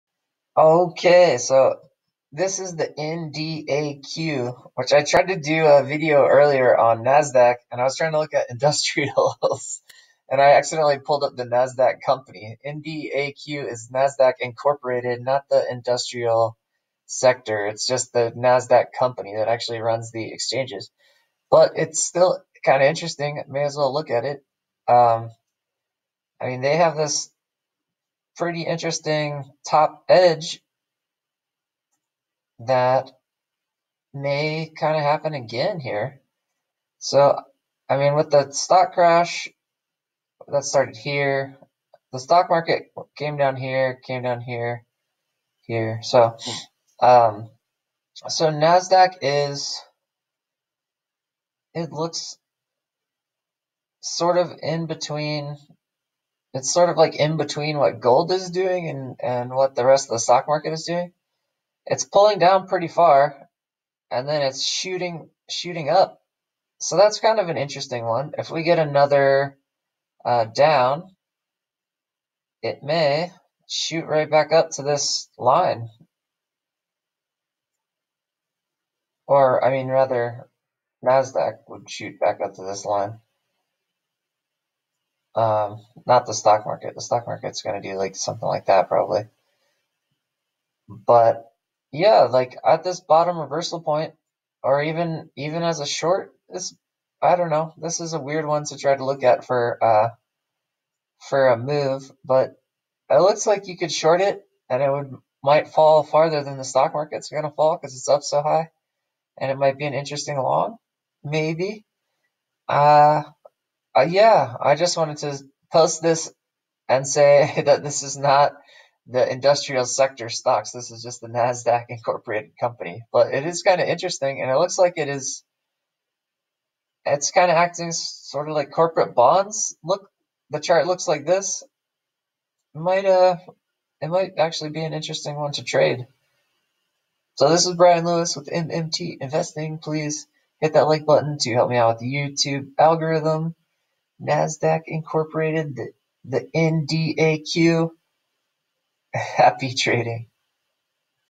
NDAQ Nasdaq Inc the exchange compay
This is Nasdaq Inc. The company that runs the stock exchanges. Not the group of industrial companies. I accidentally pulled this one up. But it is doing some major vertical moves, and worth a look for put and call options.